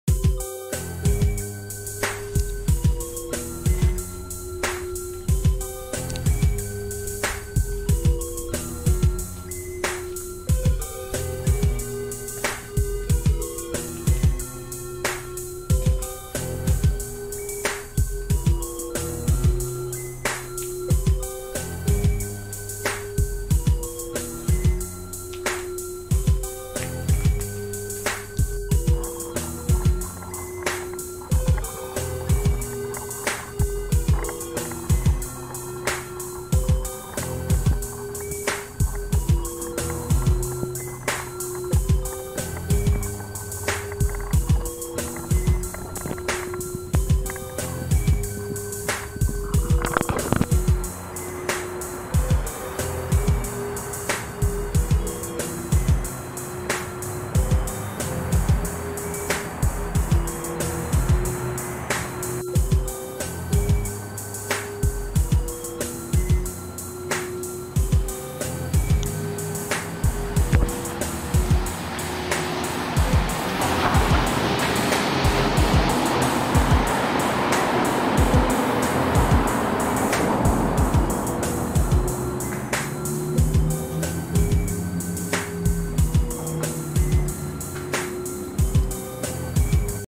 Chicago Street Gang Tags from a Decade ago -Latin Kings v Maniacs -Latin Kings v Imperial Gangsters
All compliments go to Chicago's Cold War, thank you for your work